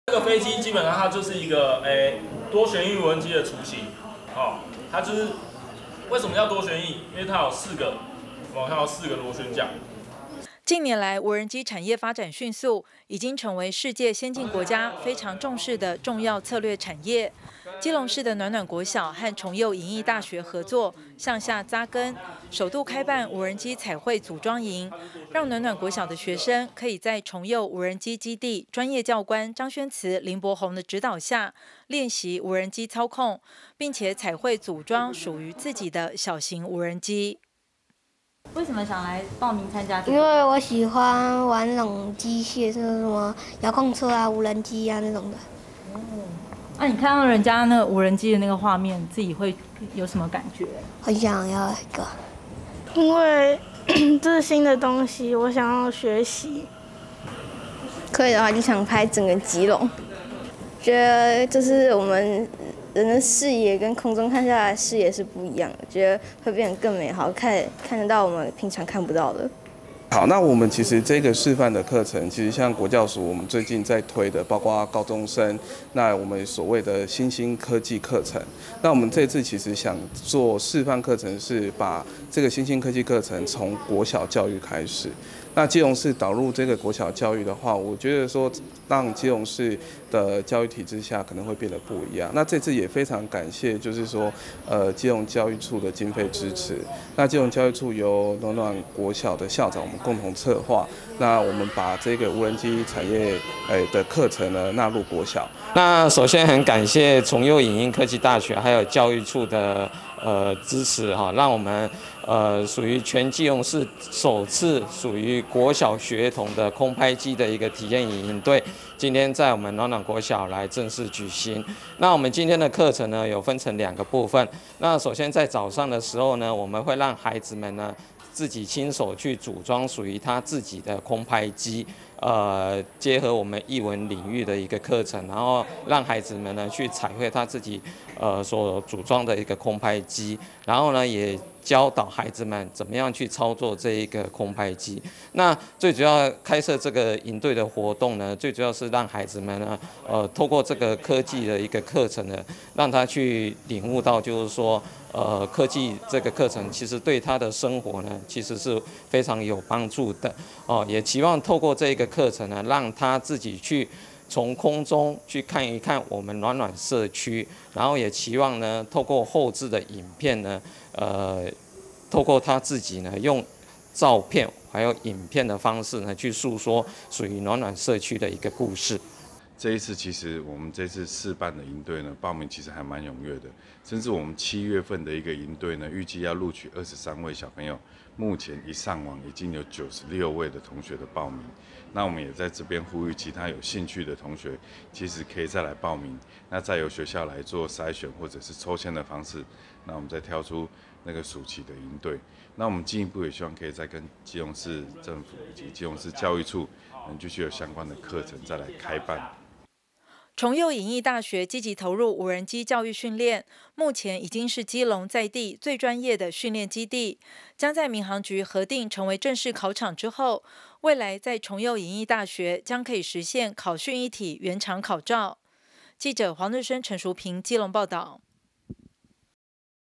崇右影藝大學與暖暖國小 首辦國小無人機研習營
(中嘉吉隆新聞記者 黃日昇 陳淑萍 基隆採訪報導)這個基隆首度舉辦的國小無人機營隊在暖暖國小進行開訓，而參加的學生都覺得非常新奇有趣，不少學生都表示，自己未來想當無人機操作飛手，拍攝更多基隆跟暖暖的影像。

近年來，無人機產業發展迅速，已經成為世界新進國家非常重視的重要策略產業。基隆市的暖暖國小就和崇右影藝大學合作，向下札根，首度開辦「無人機彩繪組裝營」，讓暖暖國小的學生可以在崇右無人機基地專業教官張軒慈、林柏宏的指導下，練習無人機操控，並且彩繪組裝屬於自己的小型無人機。

由於崇右影藝大學學校積極投入無人機教育訓練，目前已經是基隆在地最專業的訓練基地，將在民航局核定成為正式考場之後，未來在崇右影藝大學將可實現「考訓一體、原場考照」。